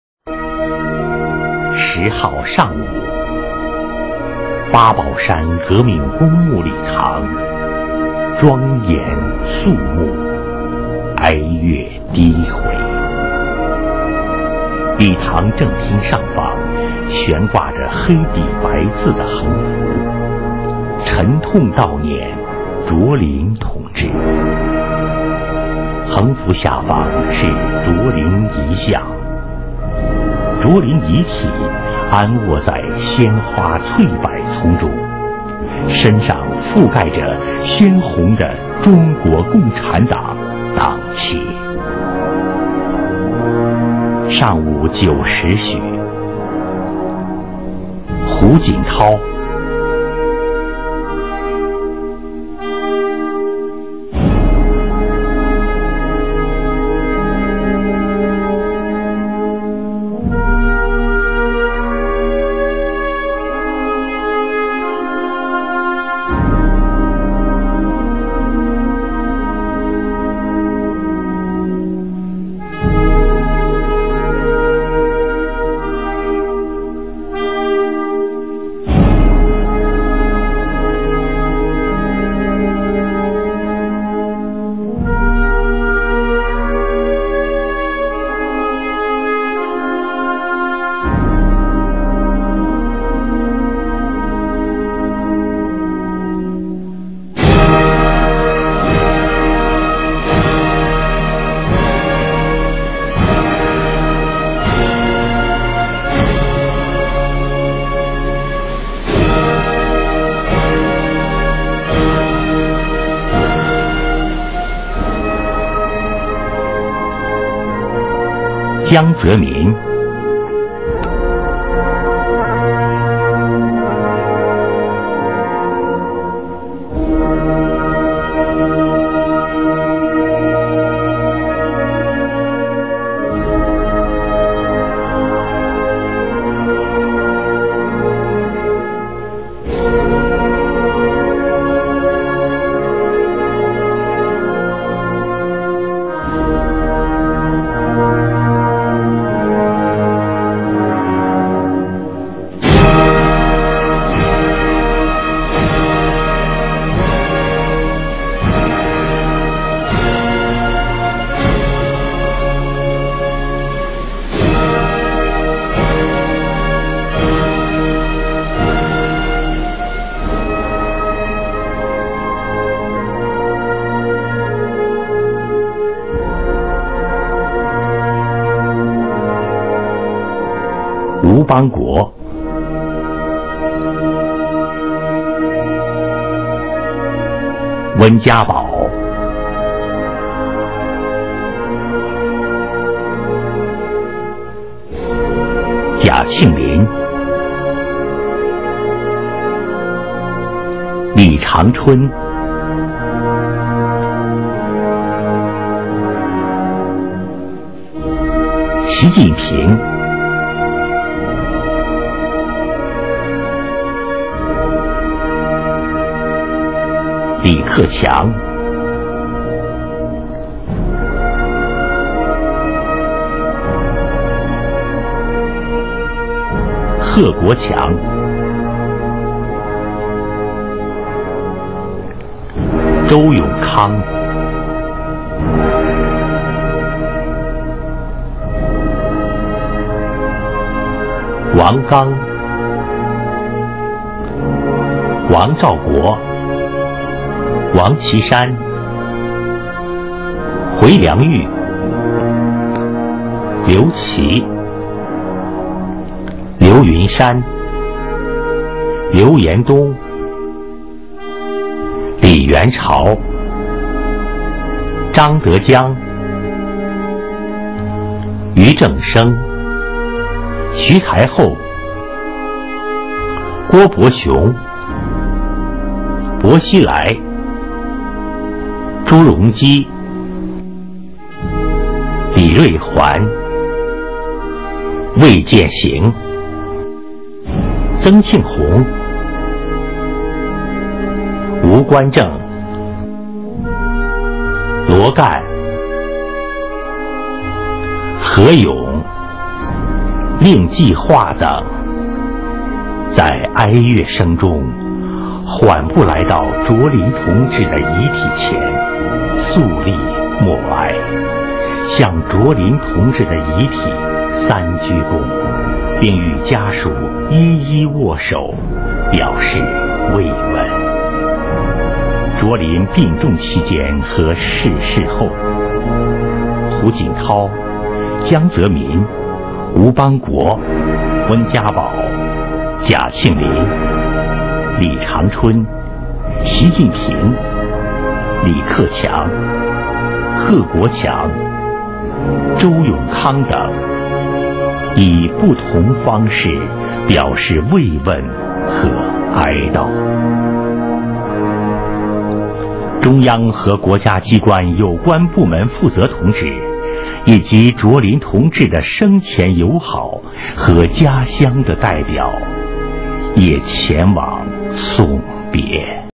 2009年8月10日，卓琳遗体在京火化，胡锦涛、江泽民、吴邦国、温家宝、贾庆林、李长春、习近平、李克强、贺国强、周永康等前往八宝山送别。等到八宝山革命公墓送别。
中国共产党的优秀党员，久经考验的忠诚的共产主义战士，中央军委办公厅原顾问卓琳同志遗体，８月１０日在北京八宝山革命公墓火化。

卓琳因病医治无效，于２００９年７月２９日１２时３０分在北京逝世，享年９３岁。

卓琳病重期间和逝世后，胡锦涛、江泽民、吴邦国、温家宝、贾庆林、李长春、习近平、李克强、贺国强、周永康等，以不同方式表示慰问和哀悼。

１０日上午，八宝山革命公墓礼堂庄严肃穆，哀乐低回。礼堂正厅上方悬挂着黑底白字的横幅“沉痛悼念卓琳同志”，横幅下方是卓琳遗像。卓琳遗体安卧于鲜花翠柏丛中，身上覆盖着鲜红的中国共产党党旗。